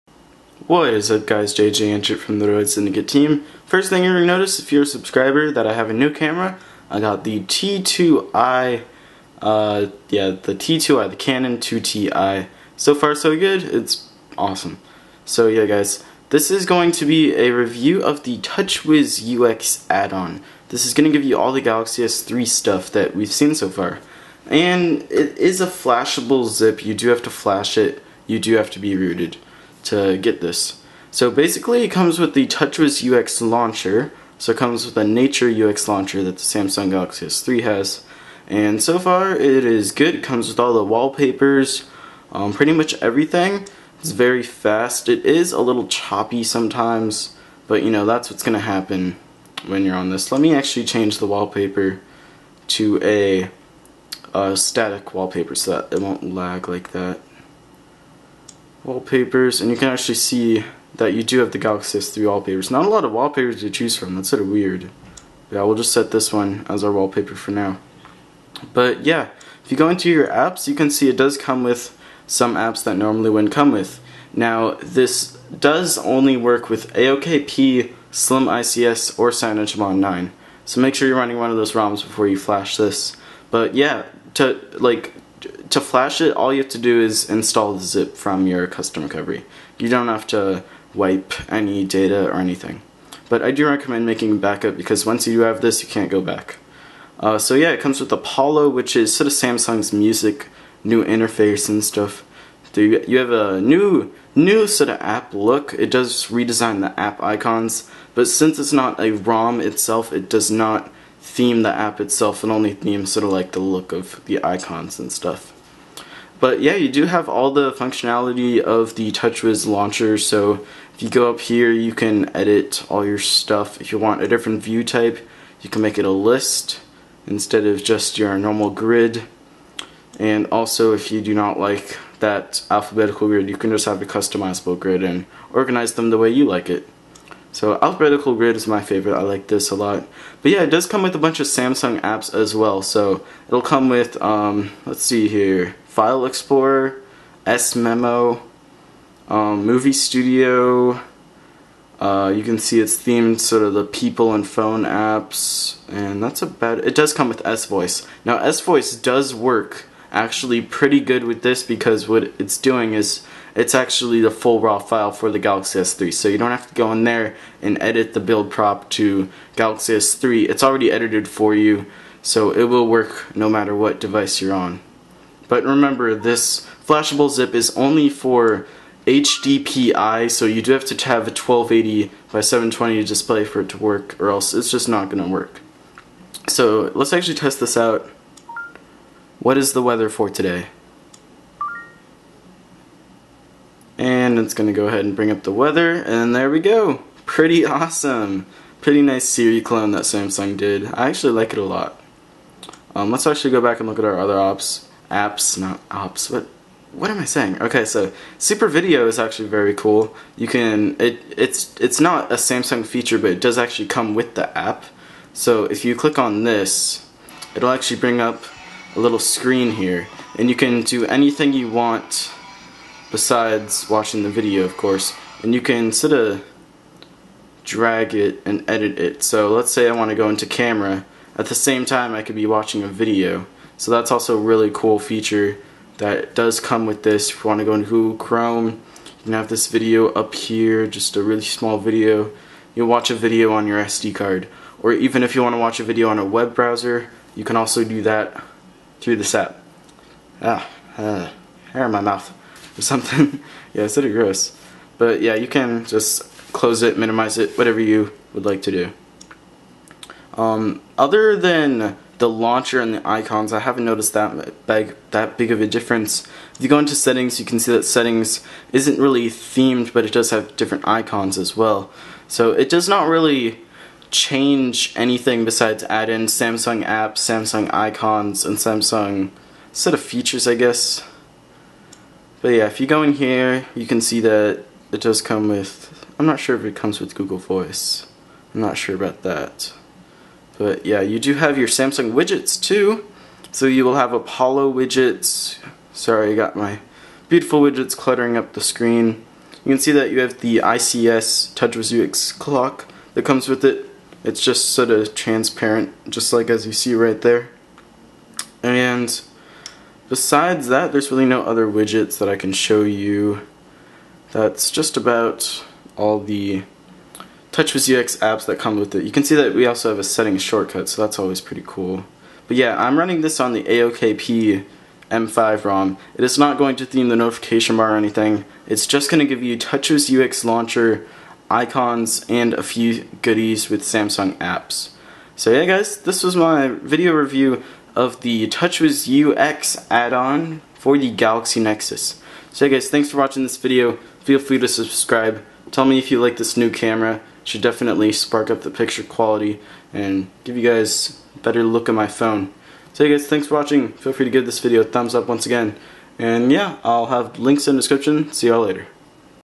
Samsung Galaxy S3 Addons! 720p Screens Only!
A Galaxy Nexus Exclusive!

This includes Touchwiz UX, Samsung icons and apps, plus a fully functional S Voice!